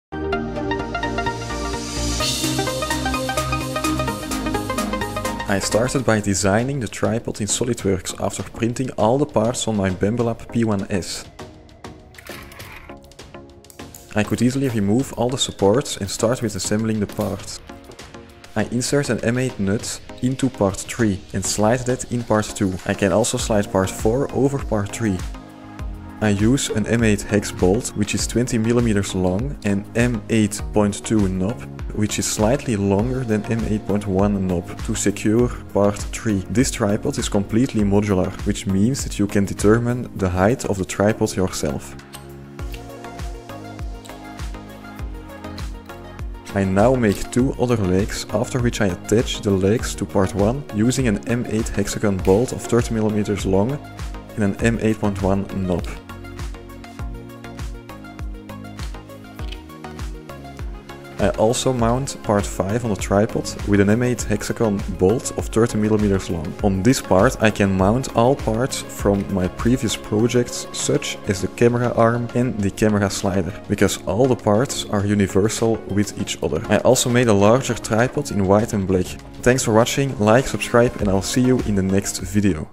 3D printed Modular Camera Tripod | Free STL Files
I made a fully modular extendable phone / camera tripod! And it's fully universal with my other camera arm and slider from a few months ago!

Timestamps:
00:00 Intro
00:04 3D Design + 3D Printing + Removing supports
00:18 Assembling
01:19 Result + Outro